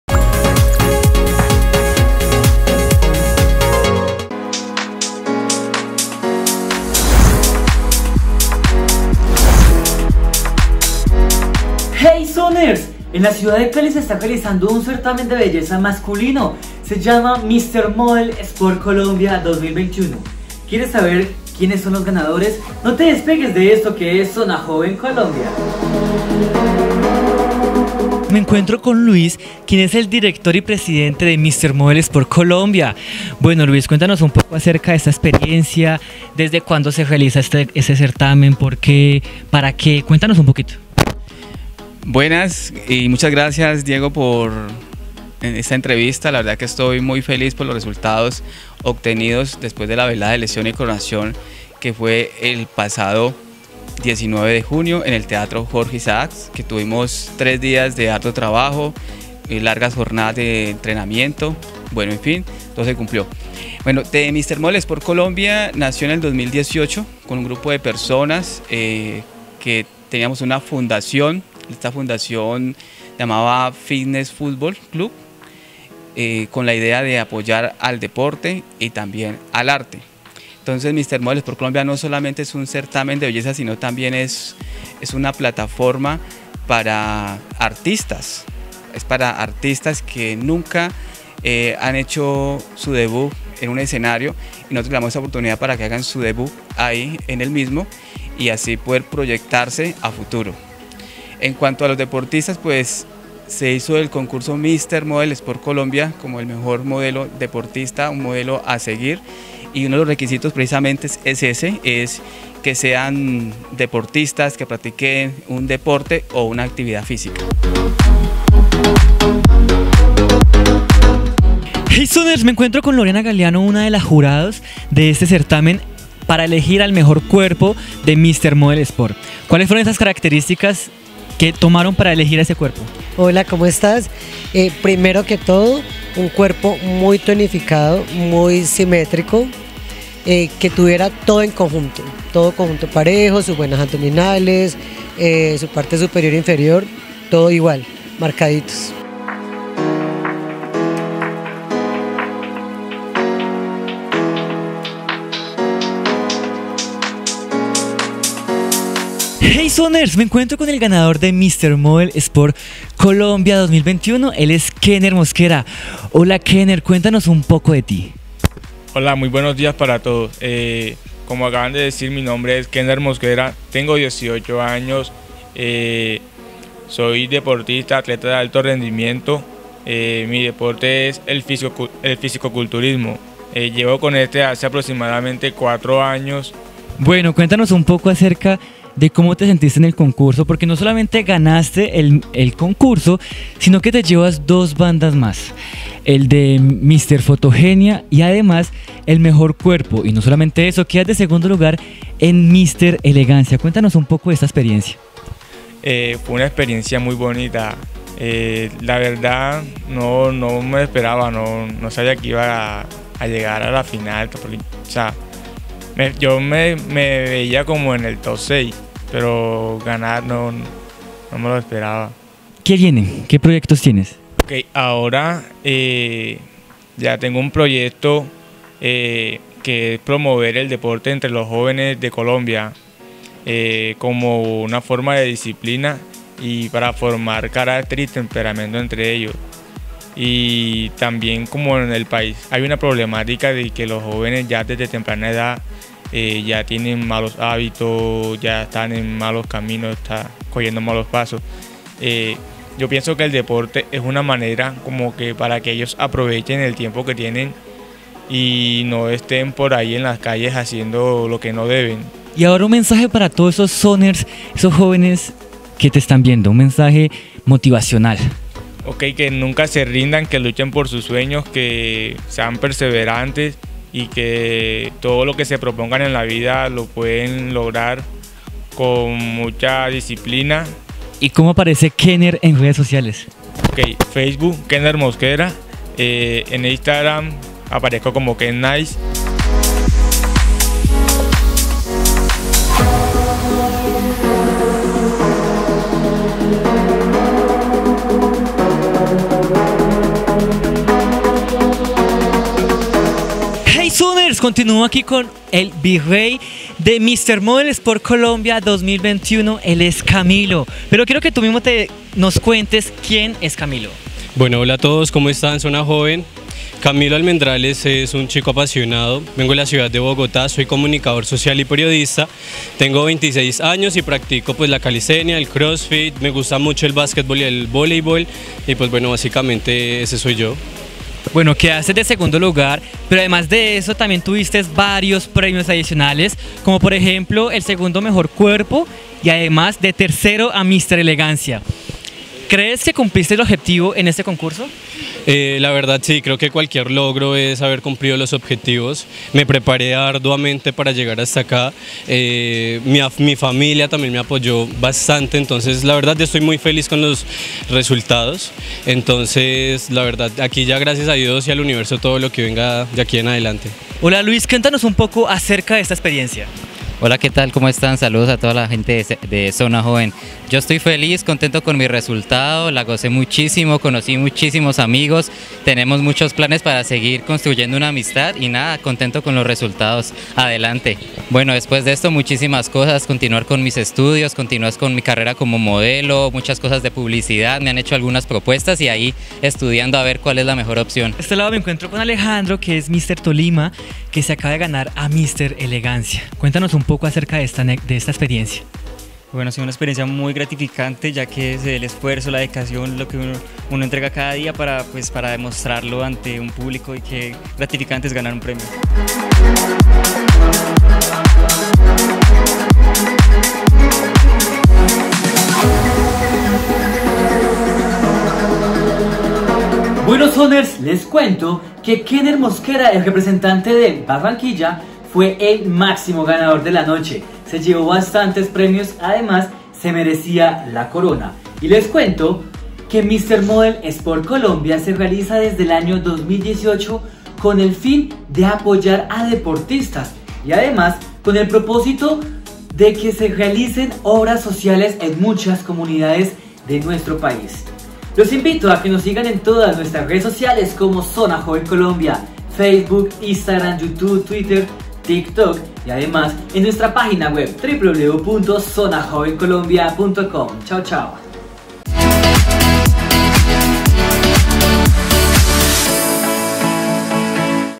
Conoce el ganador de Mr. Model Sport Colombia 2021
En la ciudad de Cali se realizó un Certamen de belleza masculino Mr Model Sport Colombia, 18 concursantes de distintas ciudades de Colombia compitieron para ser el mejor modelo deportista, y a su vez la organización busca impulsar deportistas y ayudarles con proyectos sociales en sus departamentos. Conoce el ganador y cómo se llevó a cabo este concurso.